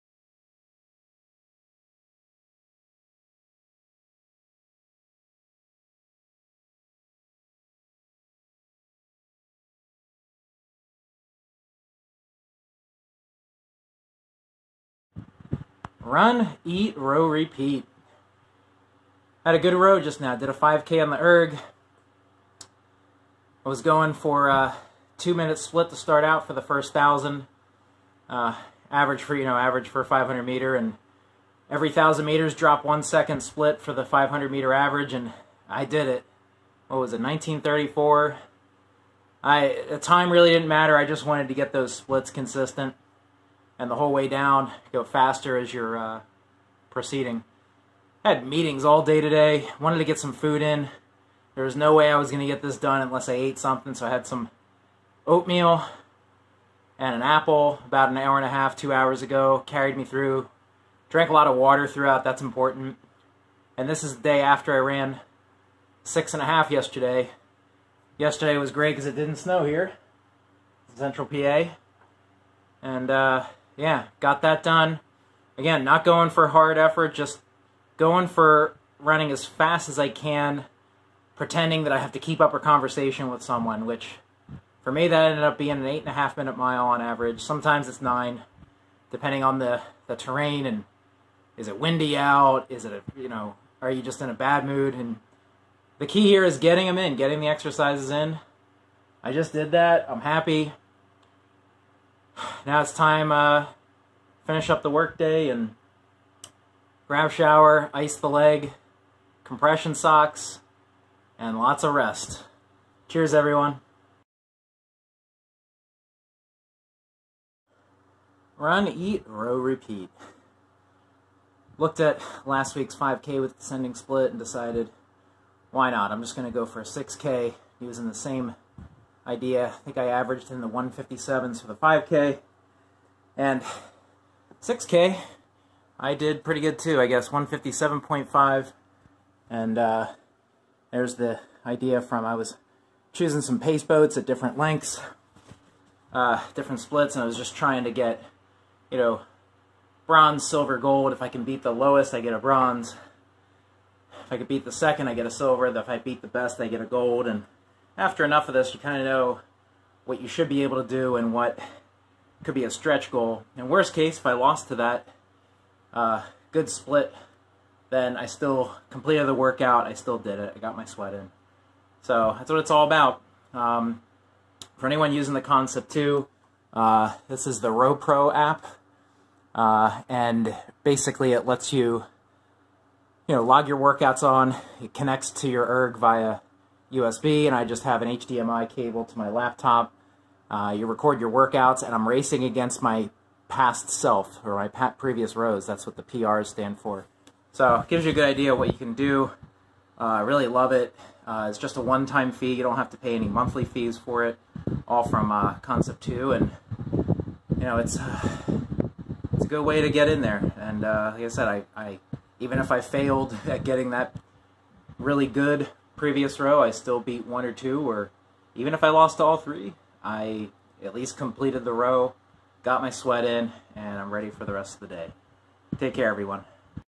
2 Ergs 5K and 6K Descending Splits
Reflections on 2 erg workouts - 5000m continuous, starting around 1:59/500m split and descending 1 second per 1000.  Same with 6000m.  Aim is to row against my previous best each time, maybe every other week or so.